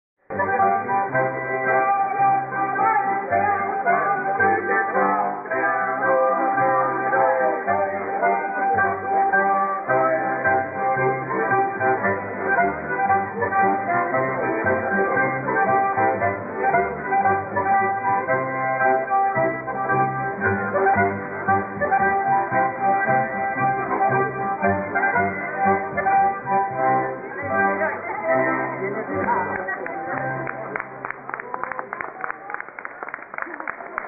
Austrian Folk Band in Salzburg
Austrian folk Band from Salzburg playing traditional folk music